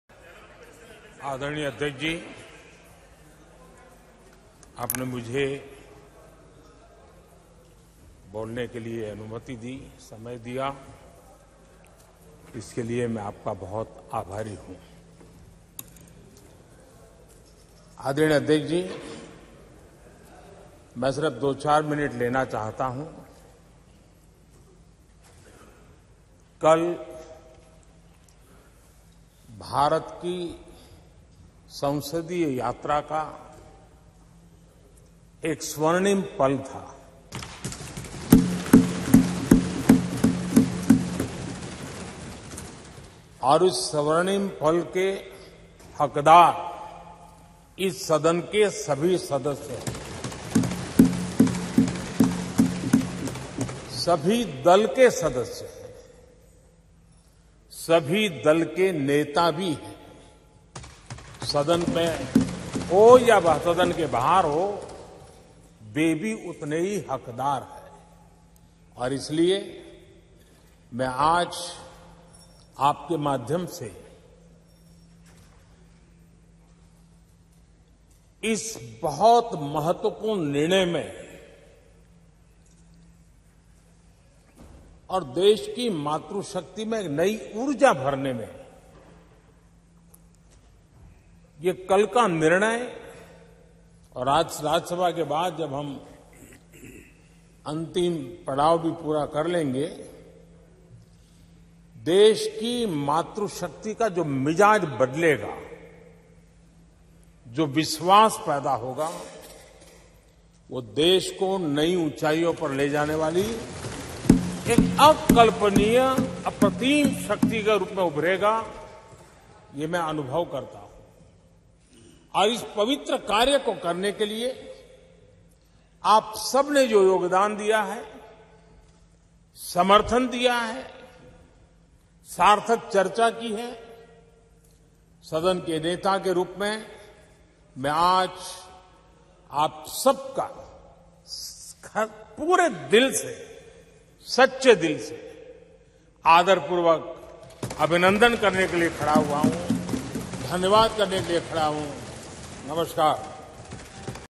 PM’s remarks on Nari Shakti Vandan Adhiniyam in Lok Sabha Wth English Subtitle
Addressing the Lok Sabha in the New Parliament Building, Prime Minister Narendra Modi held the passing of the Nari Shakti Vandan Adhiniyam as a 'Golden Moment' in the history of the Indian Parliament, attributing its success to all the MPs across party lines.